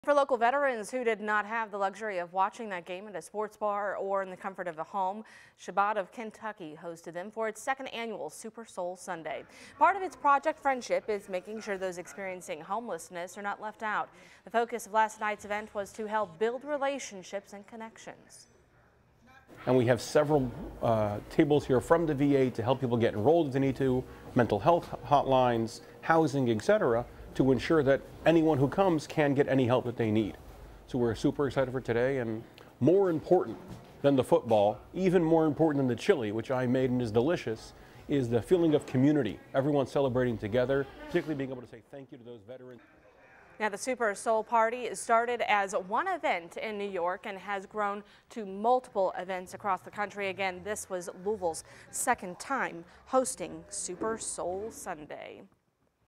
Louisville veterans enjoy 2nd annual 'Super Soul' party
Chabad of Kentucky hosted its annual Super Soul Sunday. Part of its project is making sure those experiencing homelessness are not left out.